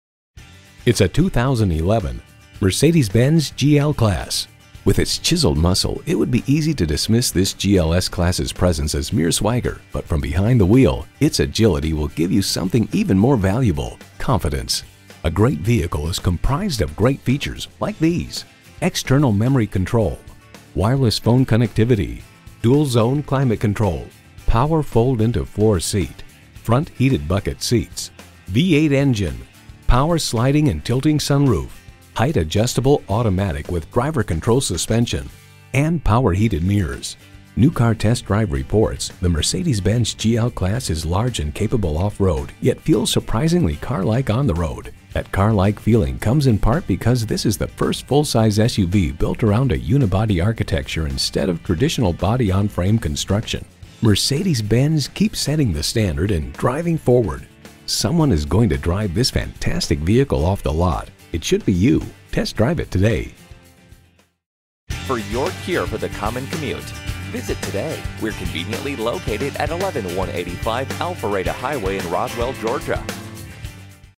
Used 2011 Mercedes-Benz GL-Class Roswell GA Atlanta, GA 5984P - SOLD
Year: 2011
Make: Mercedes-Benz
Model: GL-Class
Trim: GL 450
Engine: 4.6L V8 DOHC 32V
Transmission: 7-Speed Automatic
Mileage: 136484
Stock #: 5984P
VIN: 4JGBF7BE4BA660379
Recent Arrival!brbr2011 Mercedes-Benz GL-Class GL 450 4MATICA® 4MATICA®brbrbrReviews:br * If you are torn between a need for passenger and cargo room and a want for refined driving dynamics and appearance, the Mercedes GL-Class could be the solution for your dilemma. Those looking for fuel economy in the full-size SUV class would do well to give the GL350 BlueTEC a serious look. Source: KBB.combr * Excellent V8 performance; quiet cabin; high standard of luxury; comfortable seating for all; fuel-efficient diesel engine. Source: Edmunds

Address:
11185 Alpharetta Hwy